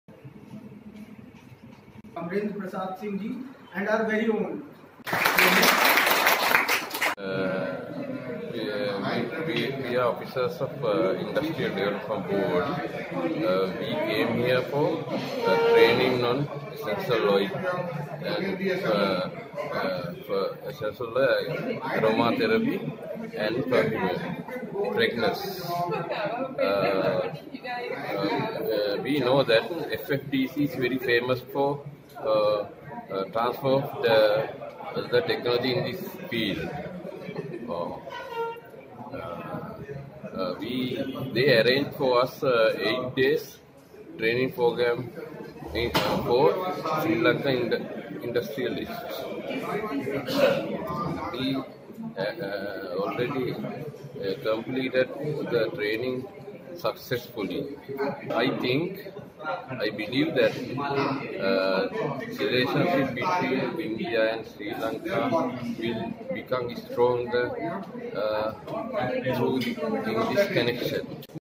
कन्नौज : इत्र नगरी की सुगंध से भारत और श्रीलंका के रिश्ते होंगे मजबूत
कन्नौज : यूपी का कन्नौज जिला इत्र नगरी के नाम से पूरे विश्व में जाना जाता है. यही कारण है कि श्रीलंका जैसे देश ने इत्र नगरी से व्यापार तक करने का मन बना लिया है. इसको लेकर श्रीलंका से आई 15 सदस्यीय टीम कन्नौज पहुंची थी, जिसने इत्र बनाने के तरीकों को सीखने के साथ ही इससे होने वाले फायदे को भी बारीकी से जाना.